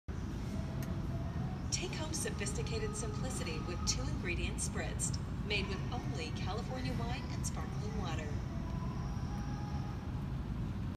In-Store Marketing Campaign for Spritzd Canada | AB InBev | powered by Tokinomo
When you implement a brand activation campaign with a retail technology solution like Tokinomo Shelfobot shoppers are going to recognize your products. It is a great way to show you are an innovative company that puts creativity first.

Spritzd from AB InBev Canada used Tokinomo Shelfobot to promote their products, create memorable experiences at the point of sale and generate awareness that goes beyond the store walls.